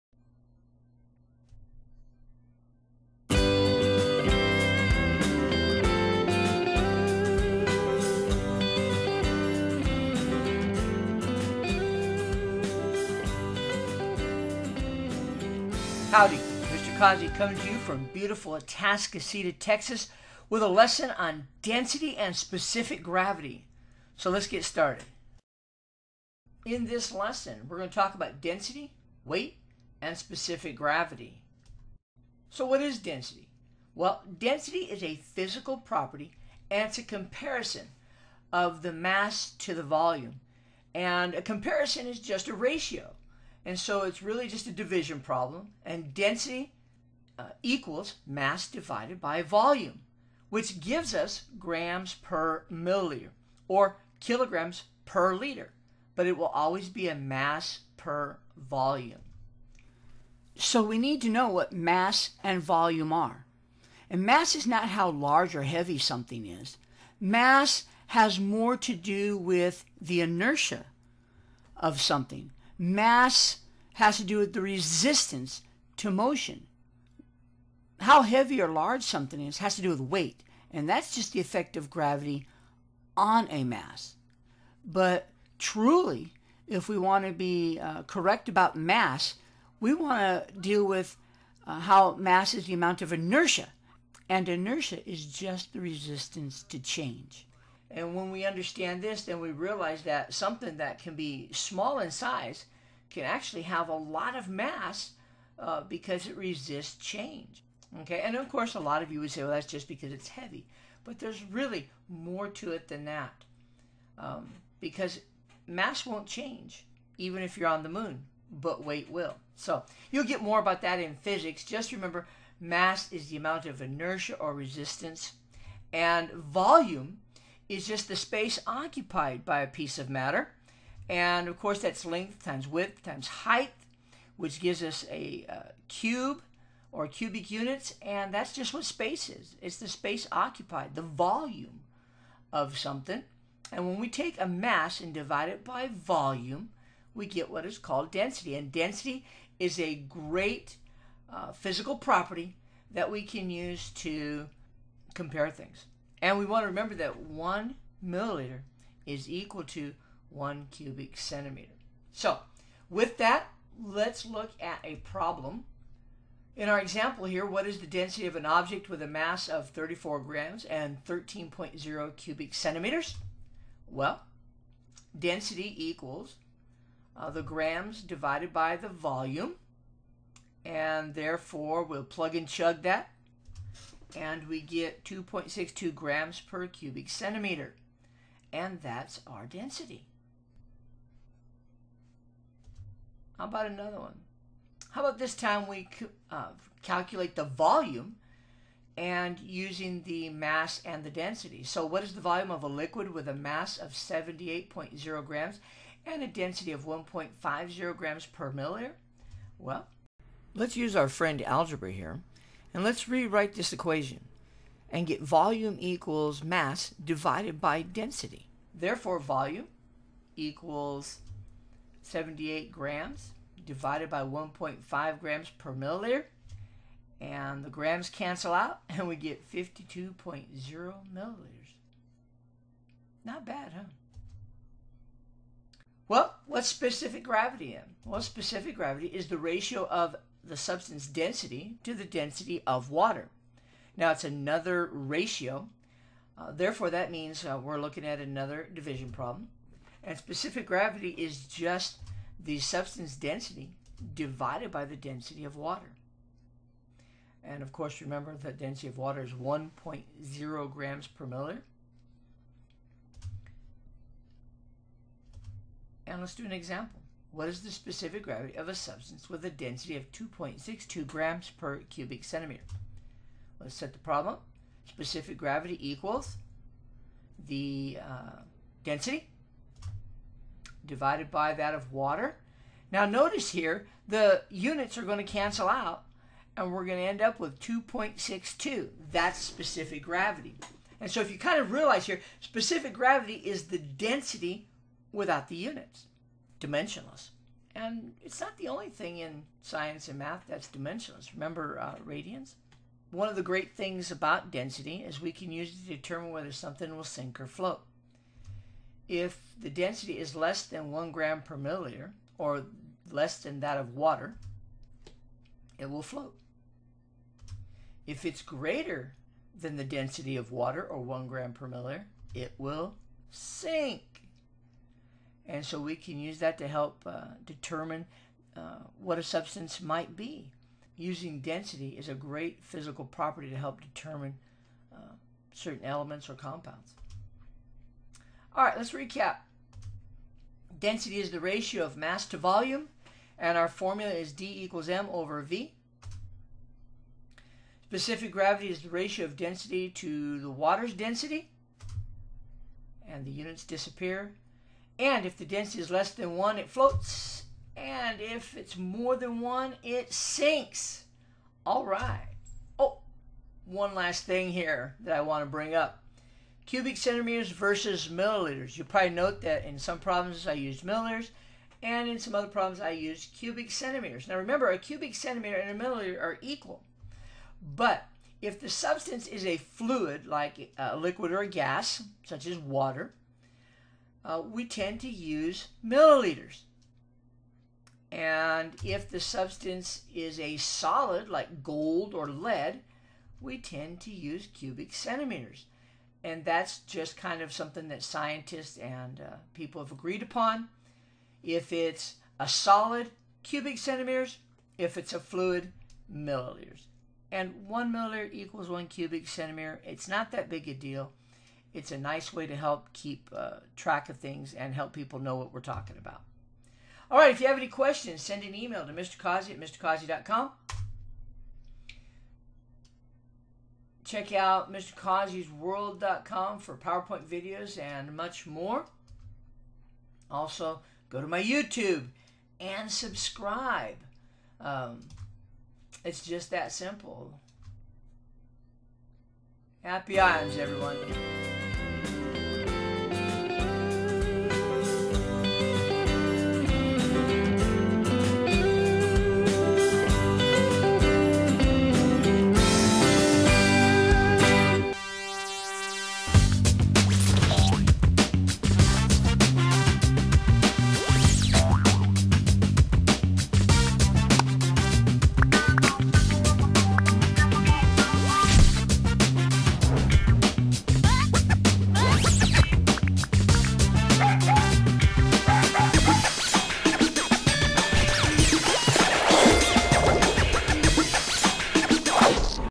Density and Specific Gravity
Density and Specific Gravity.  Mr. Causey explains density and specific gravity, the relationship between them, and how to calculate density and specific gravity.

RESOURCES:
Polyatomic Ion Cheat Sheet:

Mr. Causey explains density and specific gravity. You will learn how to calculate density and how to use density to calculate volume or mass. You will also learn what specific gravity is and how to calculate it. Happy Ions!

0:00 Introduction
0:28 IN THIS LESSON ...
0:35 DENSITY
1:00 MASS
2:06 VOLUME
2:42 EXAMPLE
3:57 SPECIFIC GRAVITY
5:07 SINK OR FLOAT
5:38 RECAP
6:57 QUESTIONS ?
7:01 CHECK IT OUT!
7:10 YOU TUBE